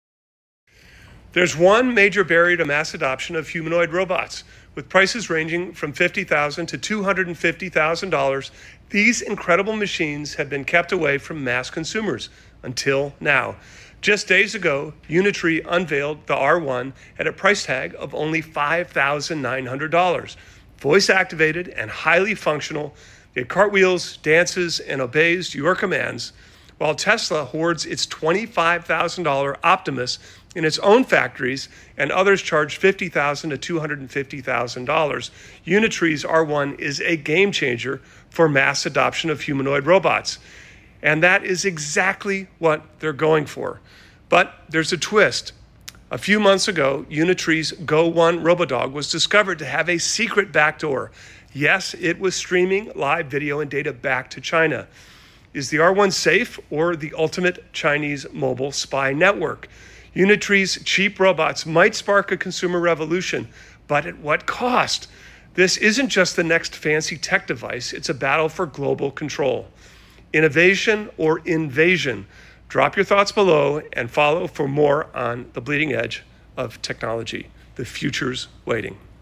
Unitree R1: $5,900 Humanoid Robot, Or A Chinese Spy?
🤖 Unitree’s new R1 humanoid launches at just $5,900 — cartwheeling, dancing, voice-activated… and possibly streaming data to China?
Innovation or invasion? You decide.
Drop your take & follow for more tech that’s rewriting the rules.